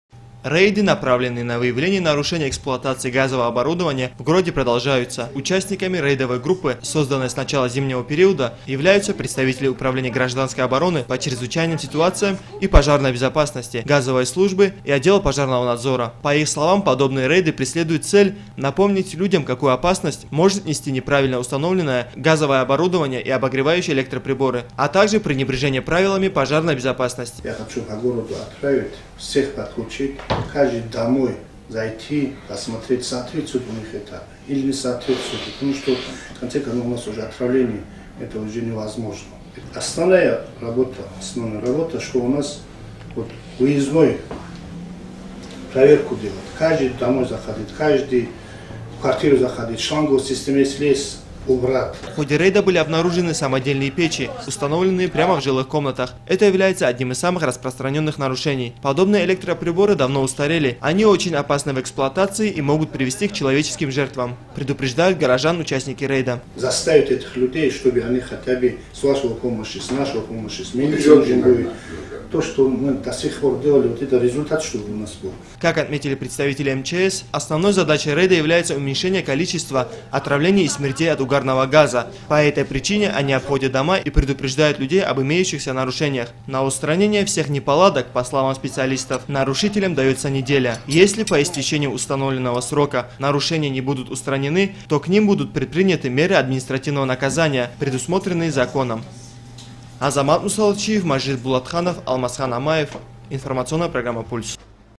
В городе прошли рейды ГО по ЧС и ПБ, совместно с представителями Отдела пожарного надзора.
На территории города Хасавюрт прошли рейды организованные управлением ГО по ЧС и ПБ, совместно с  представителями Отдела пожарного надзора.  Направленны они, в первую очередь, на профилактическую работу с населением и выявление нарушений правил пожарной безопасности. Целью рейда стало уменьшение количества отравления угарным газом.